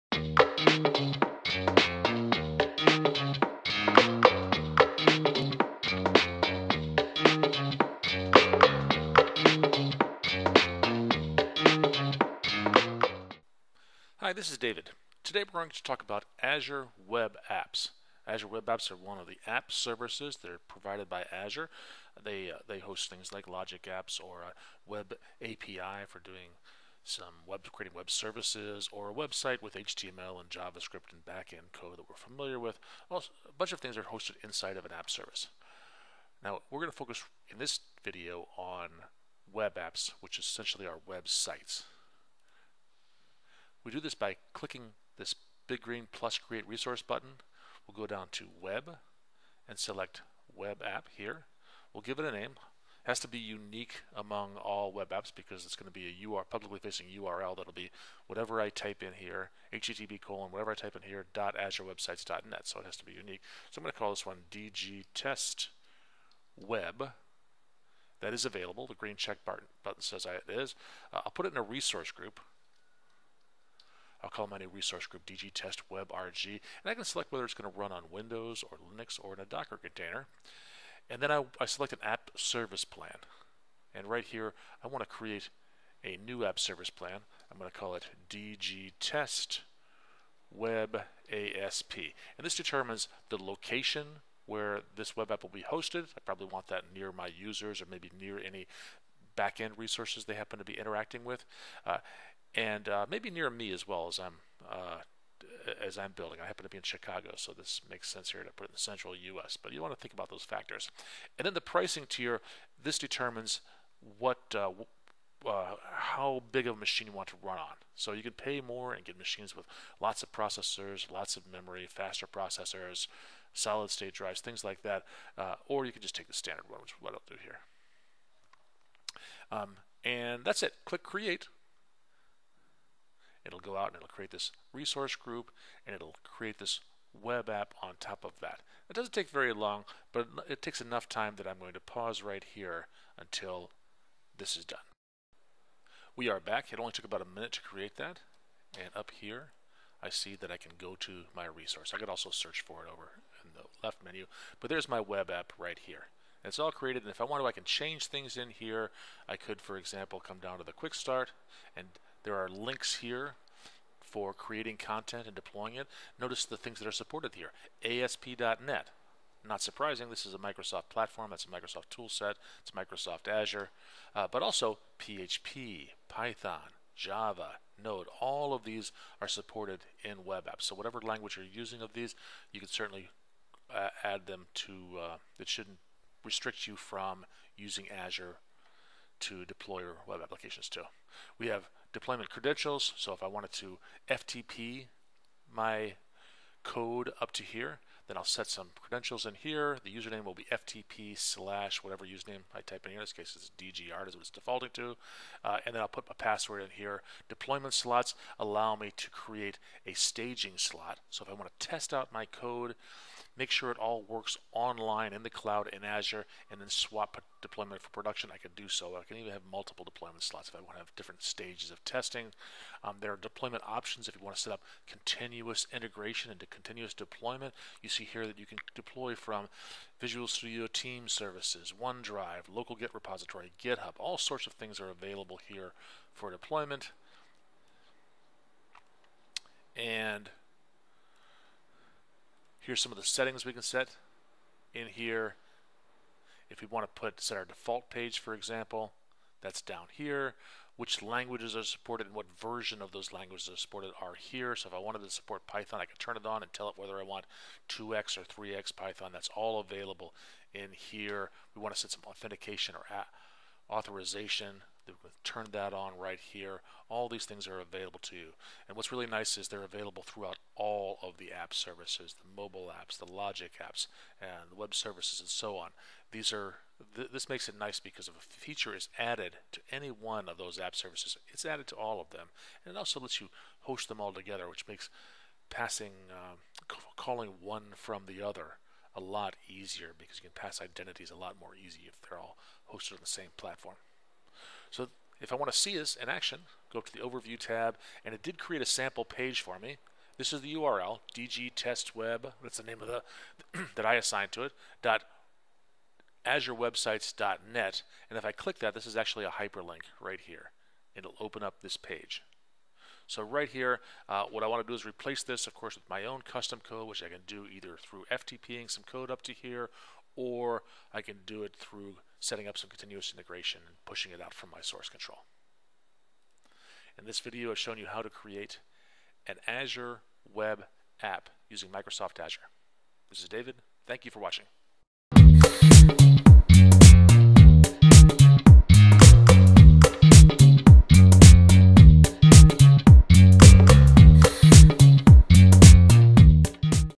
GCast 15: Creating an Azure Web App
Azure Web Apps allow you to host your web sites and applications in the cloud. I walk through the steps of setting up an Azure Web App.